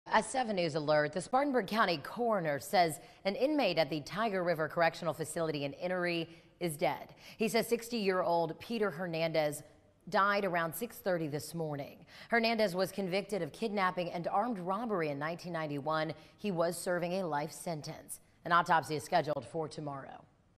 Inmate dies at Tyger River Correctional in Spartanburg Co.
SPARTANBURG COUNTY, S.C. (WSPA) – An inmate died early Thursday morning at Tyger River Correctional Institution in Spartanburg County.